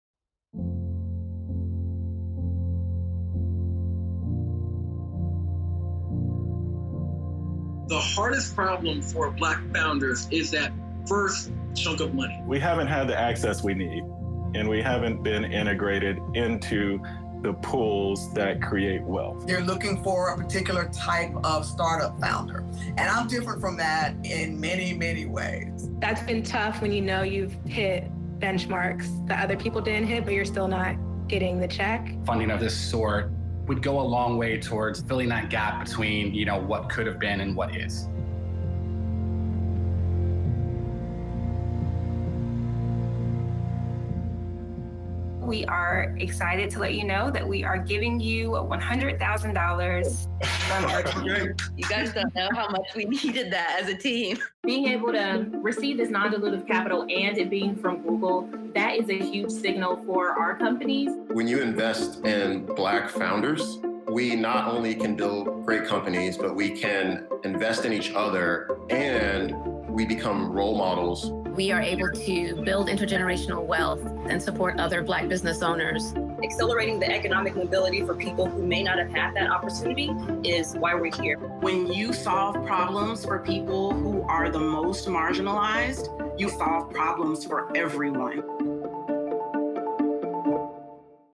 Funding Black startup founders fuels generational change
Across the U.S., incredible Black founders building promising startups are consistently locked out of access to capital that is critical to growing their businesses.

The Google for Startups Black Founders Fund will provide hands-on support and up to $100,000 each in non-dilutive funding to inspiring Black startup founders across the country.